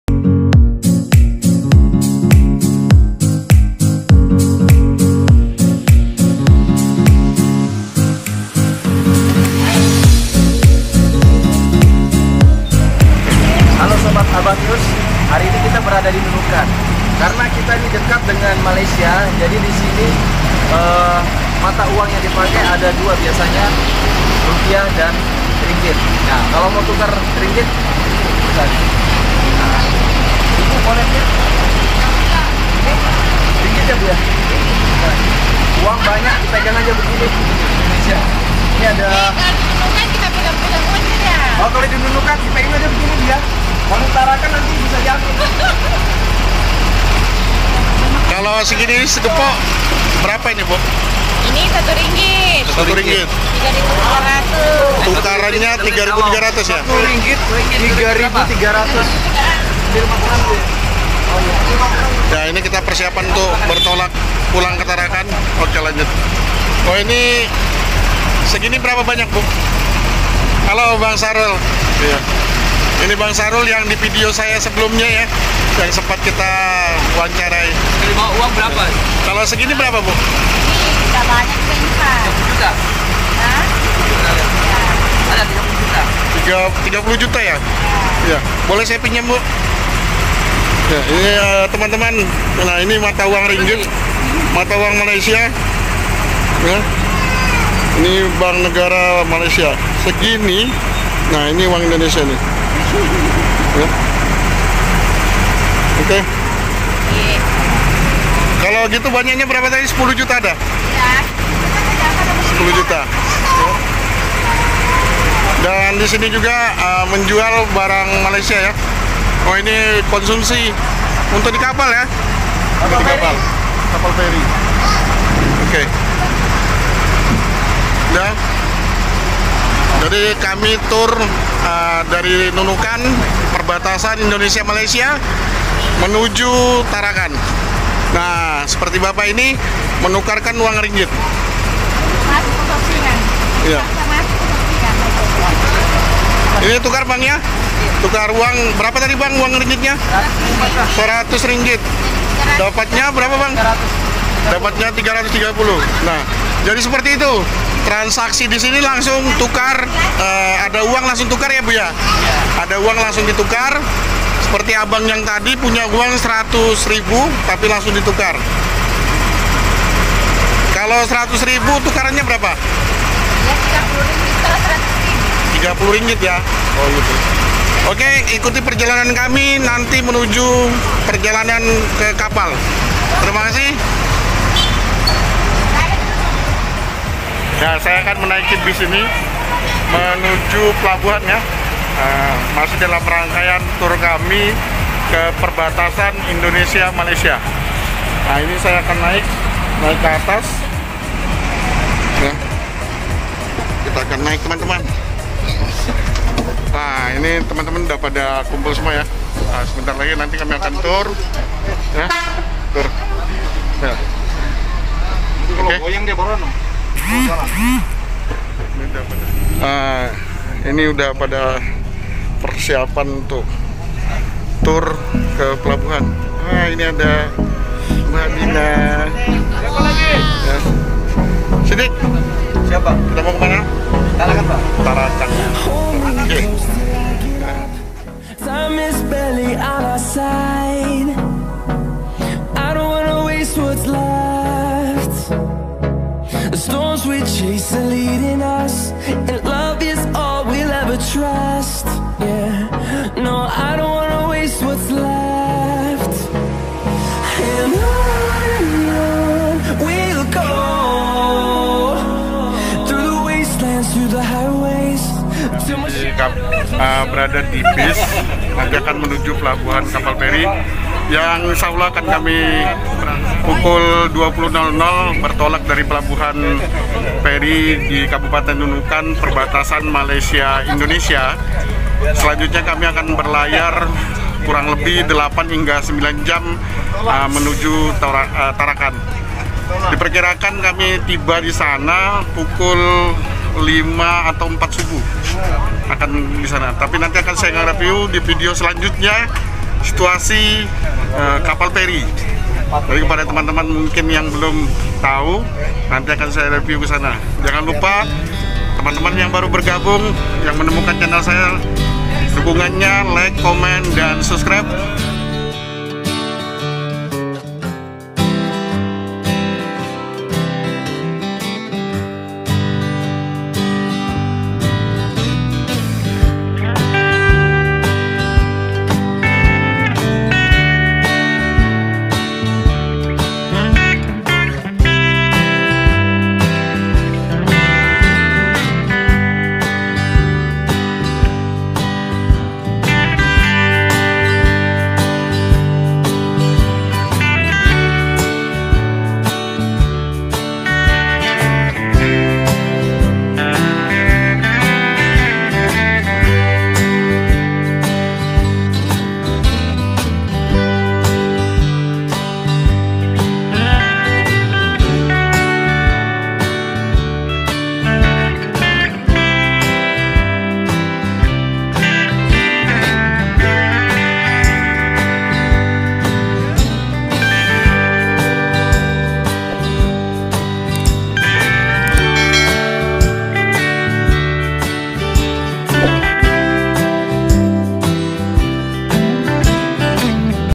Di Saat Pulang bertemu dengan Istrinya Bang Sahrul yang berprofesi sama‼️Bisnis Penukaran Uang Asing
Assalamualaikum Gooi

selamat datang di Channel Abang Iyus
bagaimana kabar sahabat semua semoga dalam keadaan sehat wal'afiat Aamiin ya robbal Aalamin..

video kali ini yaitu di saat abang iyus bersiap bertolak berangkat dari Nunukan menuju Tarakan..bertemu dengan ibu ani isyrinya bang Sahrul yang berfrovesi sama dengan bang sahrul yaitu bisnis penukaran uang asing ,di saat itu ibu ani sedang duduk di pinggir jalan bersama bang sahrul..

bagaimana keseruannya simak video selengkapnya di cahnnel Abang Iyus.

wassalamualaikum waromatullohi wabarokatu